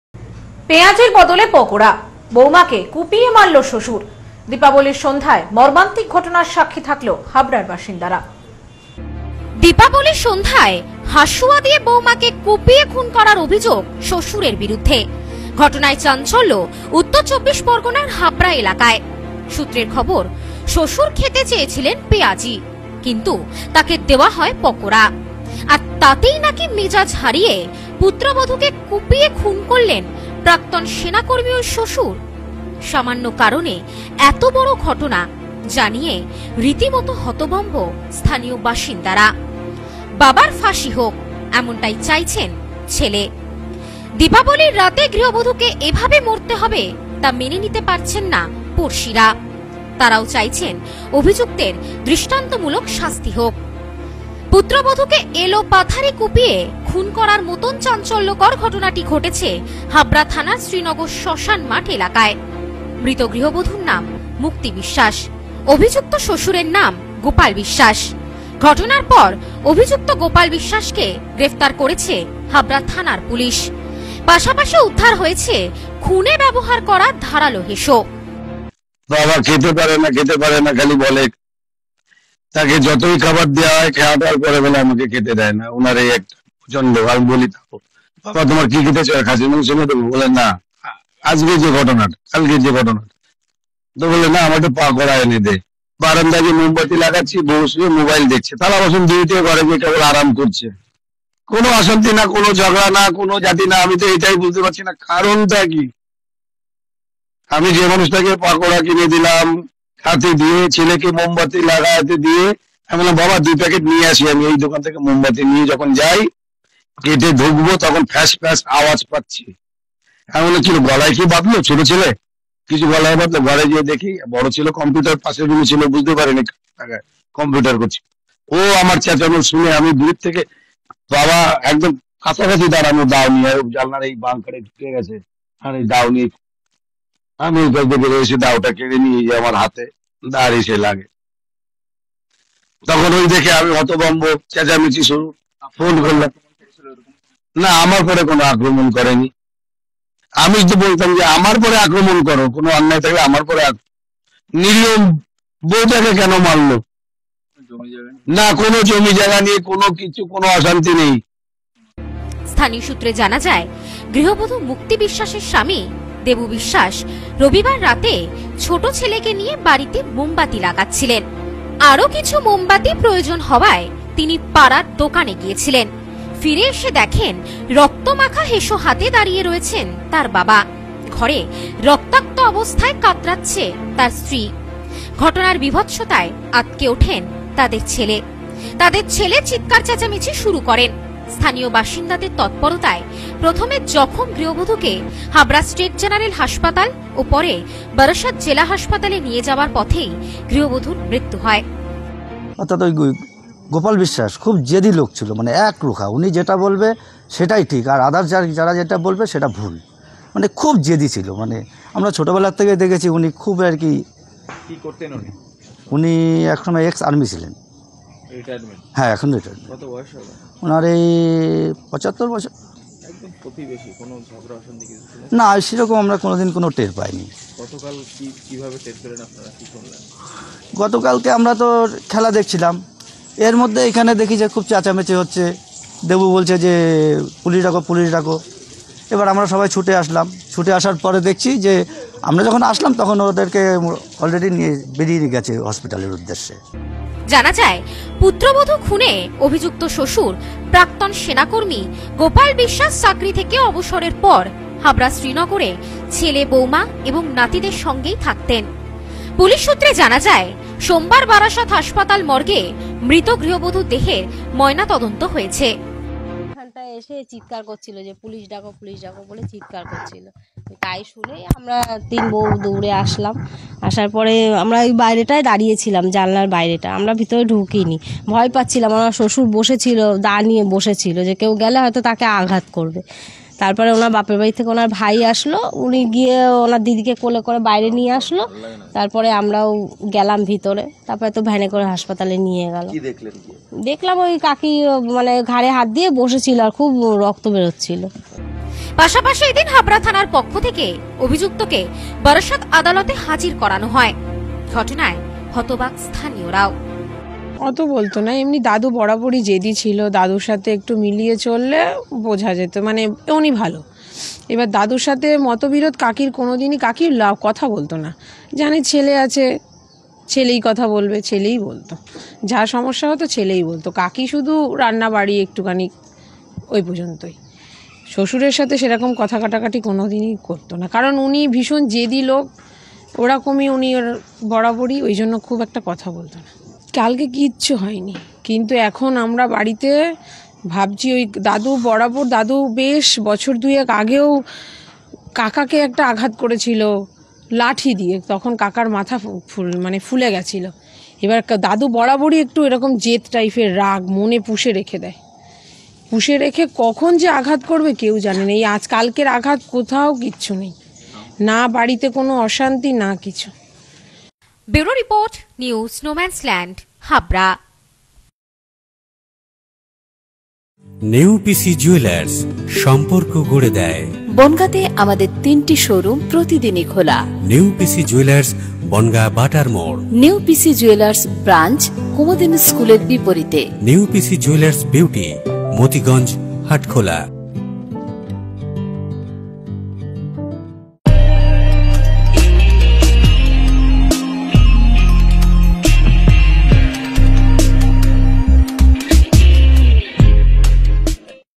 পেঁয়াজির বদলে পকোড়া! বৌমা কে কুপিয়ে মারল শশুর।
পেঁয়াজির বদলে পকোড়া! বৌমা কে কুপিয়ে মারল শশুর। দীপাবলীর সন্ধ্যায় মর্মান্তিক ঘটনার সাক্ষী থাকলো হাবড়া এলাকার বাসিন্দারা।